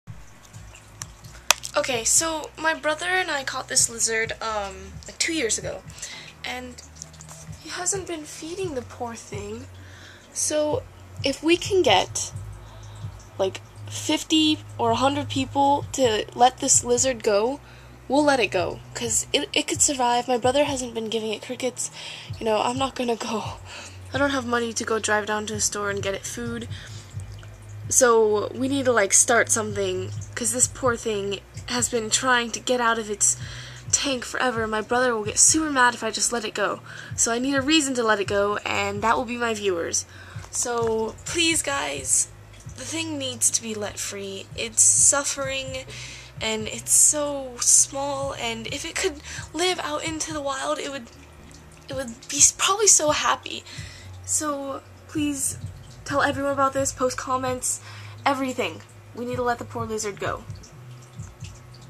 LET SIPHER(the lizard) GO!!!!!!!!!!!!!!!!!!!
My brother caught a lizard and has been taking very horrible care of it.
Please comment, and get people to comment, telling my brother to free the lizard!

If we get 50-100, he might do it!